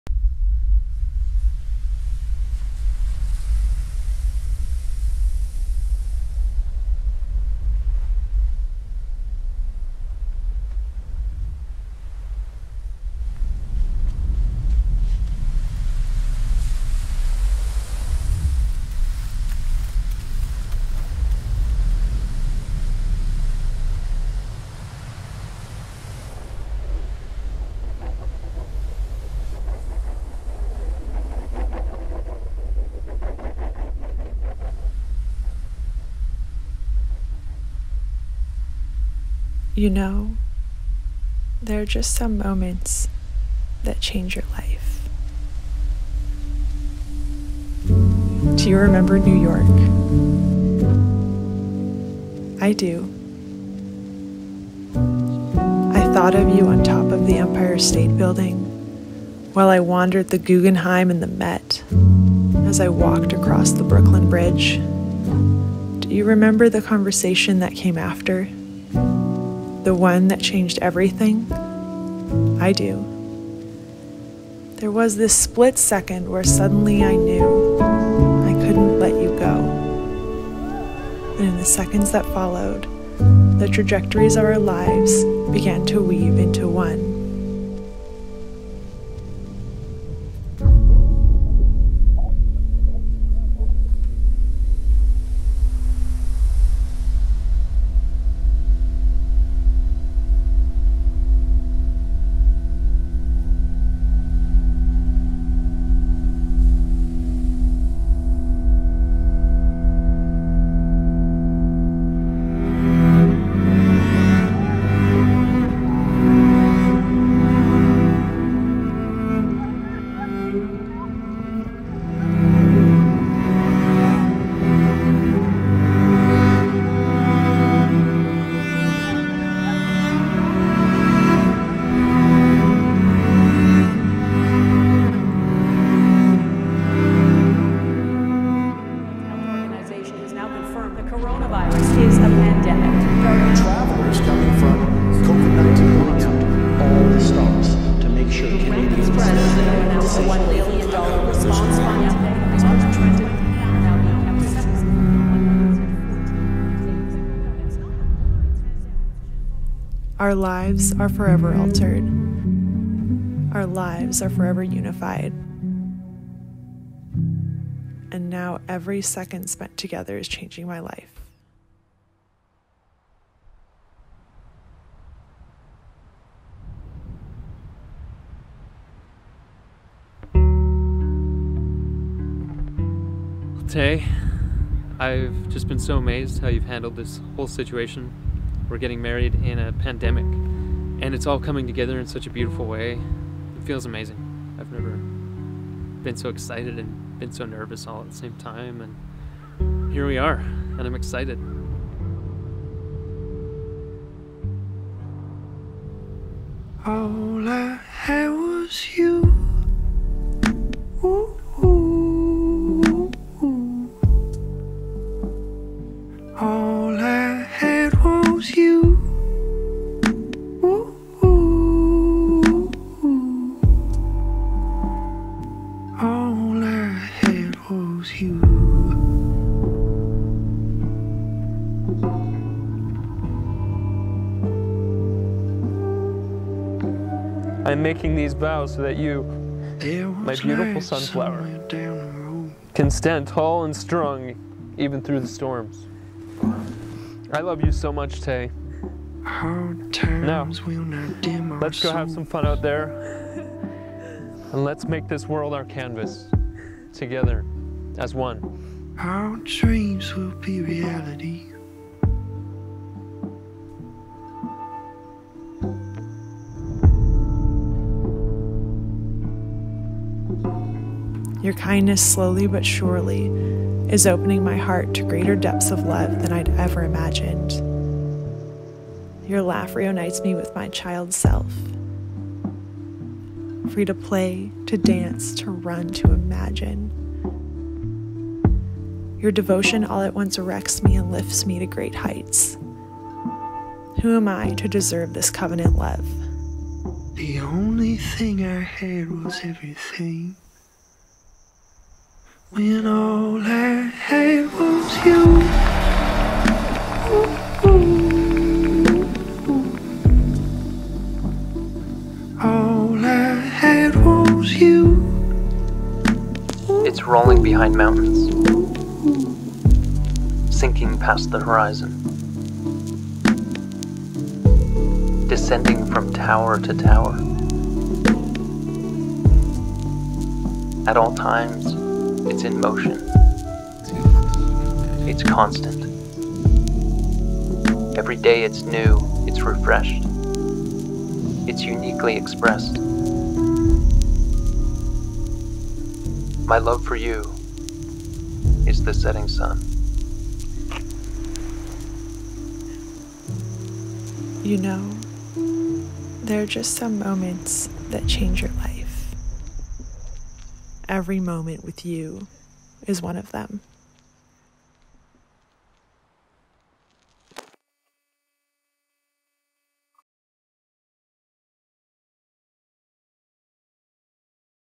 Nathan + Tayllor | The Story
From dating to marriage. Our story told by Adam Combs.